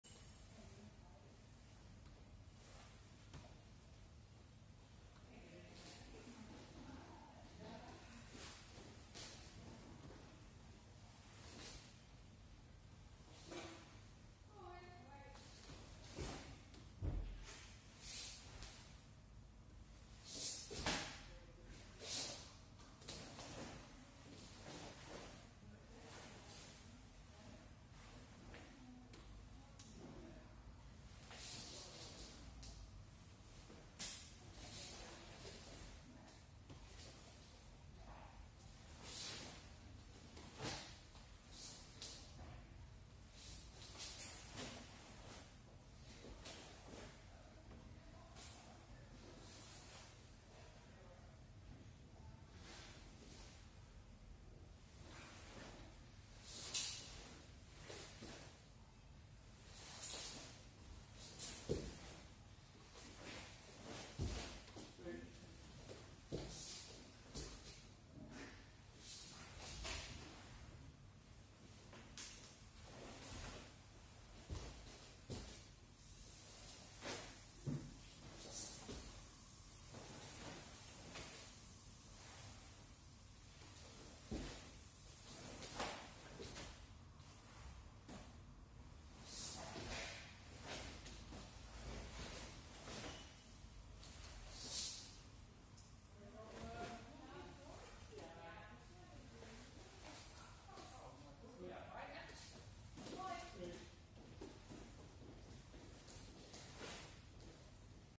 Nipaipo andere camerahoek | dojo | Kaihatsu Karate-do
Nipaipo, opgenomen met een andere startpositie. Gefilmd om alle bewegingen goed in beeld te krijgen. Dit is Nipaipo zoals we die oefenen bij Kaihatsu Karate-do, de karateschool voor Burgum en omstreken. Deze uitvoering toont vooral de stappen en technieken. Ik toon hier nog niet de nuances in het ritme.